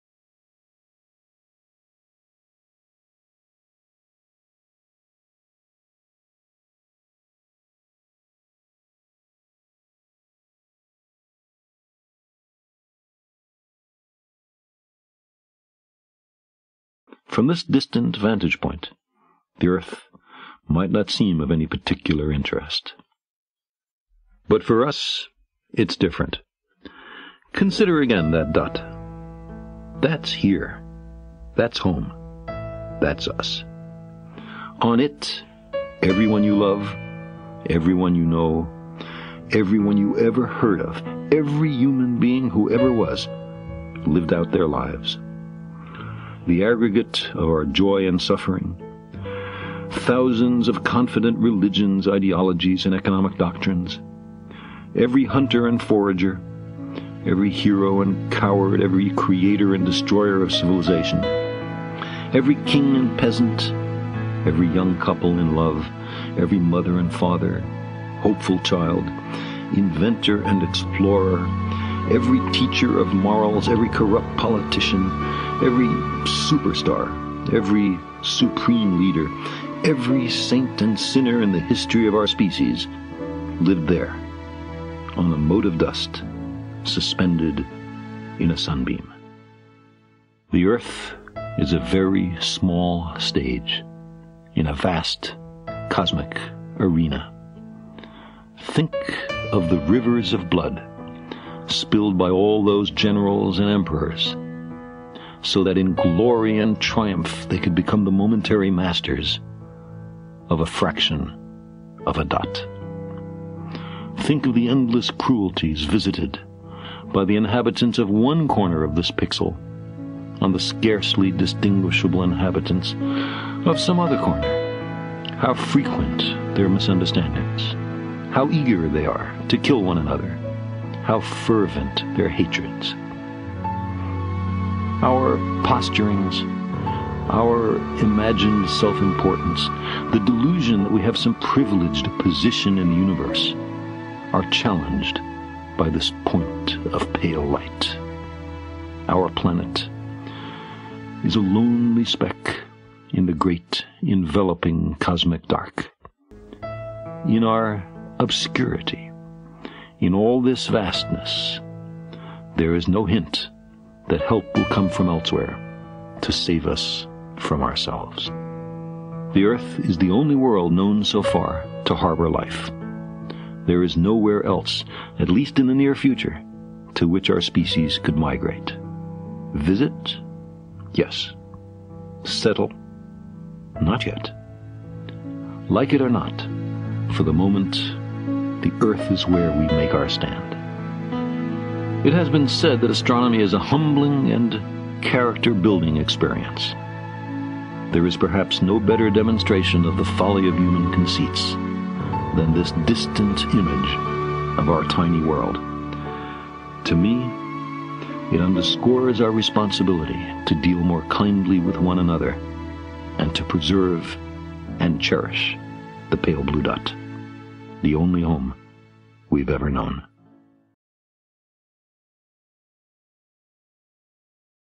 Carl Sagan - Pale blue dot (Subtítulos en español)
Valiosa reflexión del astrónomo Carl Sagan del libro "Pale blue dot"

Realizado por laloDT7799